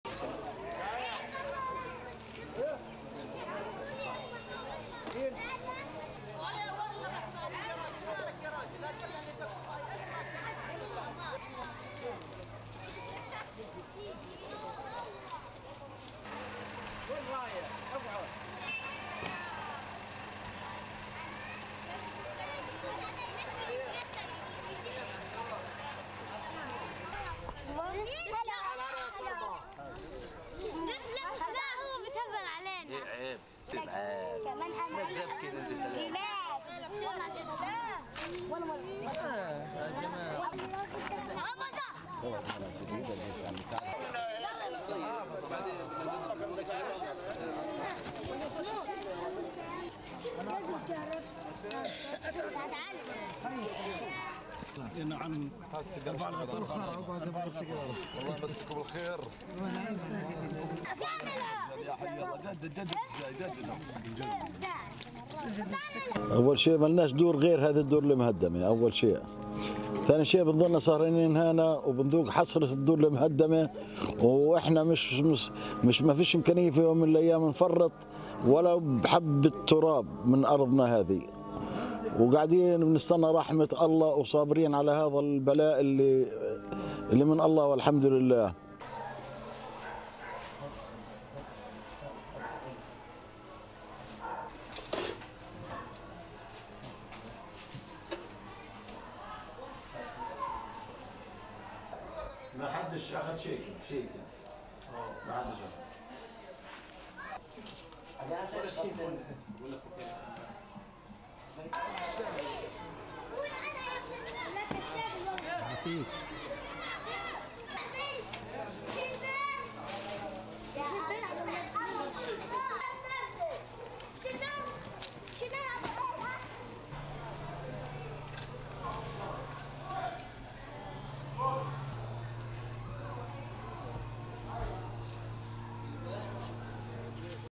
Gaza family's precarious living amidst rubble
(28 May 2021) A family from Gaza City huddled around a fire surrounded by the ruins of their home.
The demolished multi-story apartment buildings around them, which housed eight families, reveal the destruction of the latest round of fighting between Hamas and Israel.
"We stay up here next to our destroyed homes and bemoan it. And we cannot one day, give up a single grain of dirt from our land," said Jamal Nseir, the owner of one of the destroyed homes.
"We are sitting here waiting for the mercy of God," he added.
The 11-day Gaza war killed more than 250 people, mostly Palestinians, and caused widespread destruction in the impoverished coastal territory.